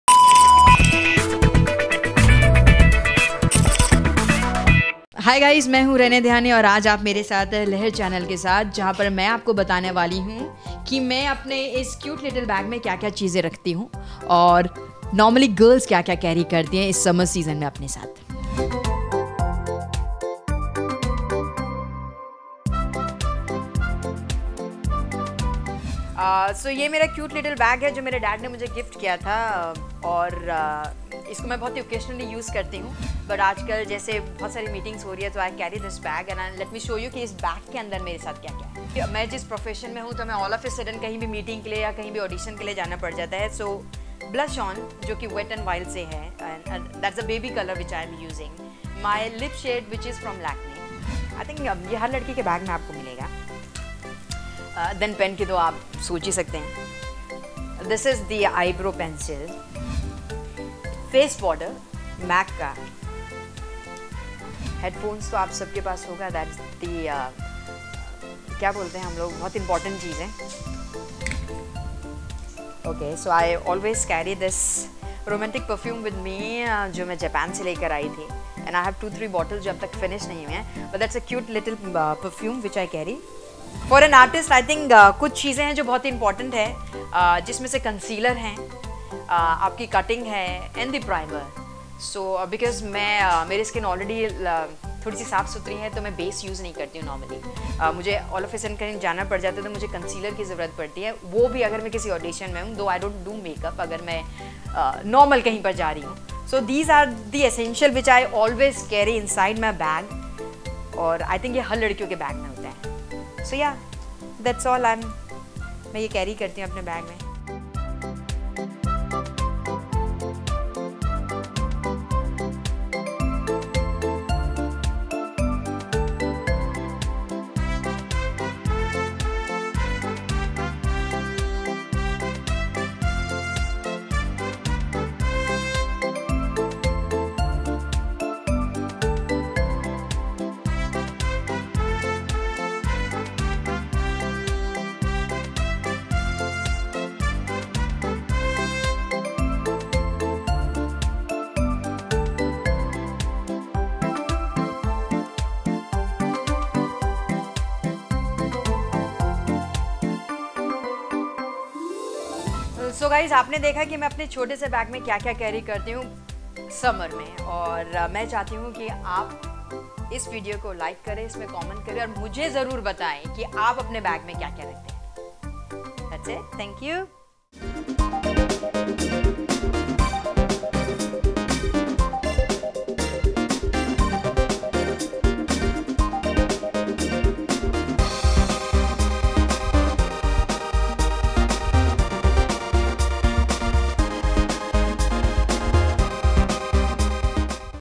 Inside My Bag With Renee Dhyani | Exclusive
Watch! the special segment Inside My Bag with Renee Dhyani. The TV actress gave special sneek peak inside her bag.